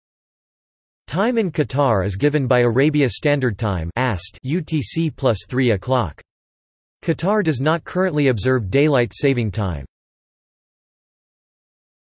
Time in Qatar | Wikipedia audio article
This is an audio version of the Wikipedia Article:
Time in Qatar

- Socrates

SUMMARY
Time in Qatar is given by Arabia Standard Time (AST) (UTC+03:00). Qatar does not currently observe daylight saving time.